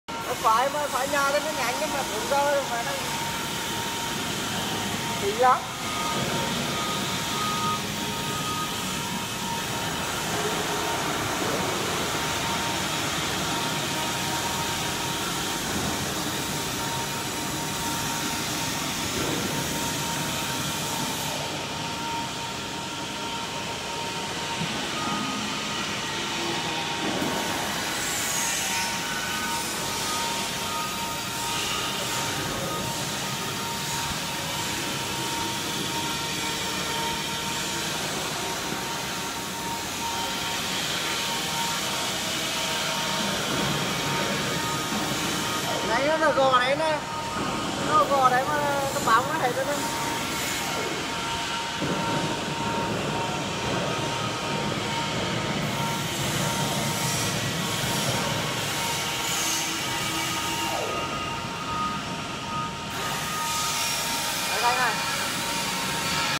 Visual Inspection for HDG Plates
Visual Inspection for HDG Plates, video record the visual inspection of knee flip plates after HDG